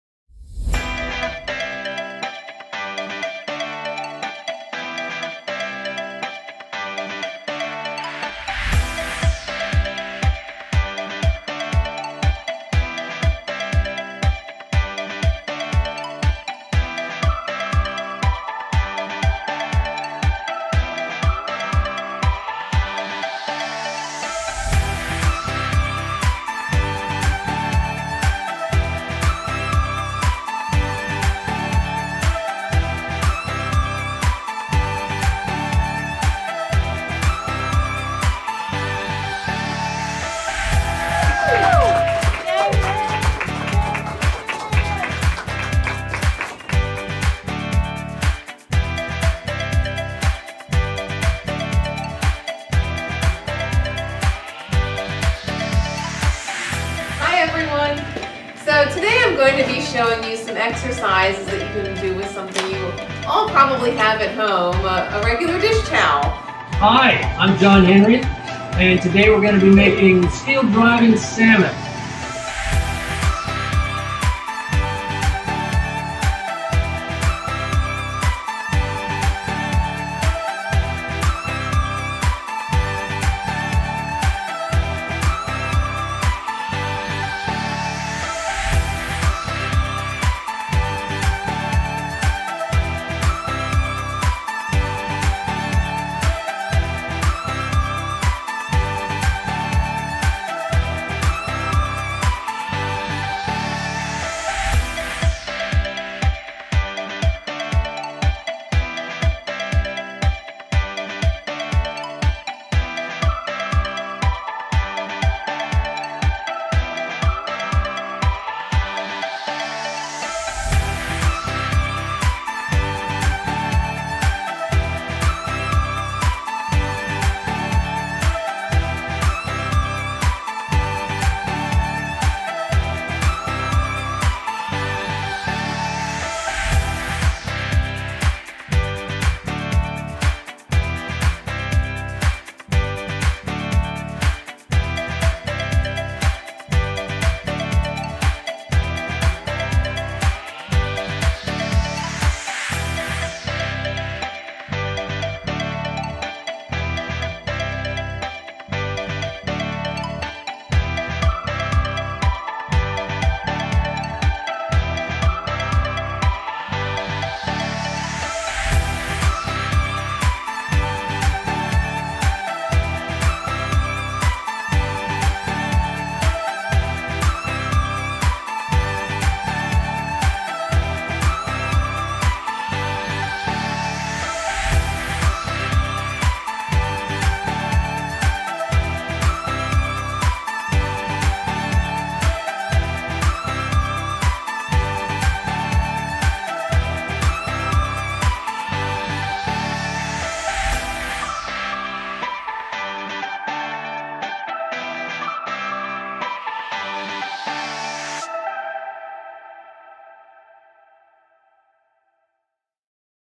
COVID-19 - Supporting Residents and Team Members
Since March 2020, COVID-19 changed how the Lutheran Social Ministries of Maryland communities operate, to reduce the spread of this pandemic. At the end of our fiscal year, June 2020, we reflected on challenges and rewards. Though COVID-19 has been challenging, we feel especially rewarded by  our team members' dedication and hard work to keep residents safe and engaged as well as the support they and the local communities have provided to our team members.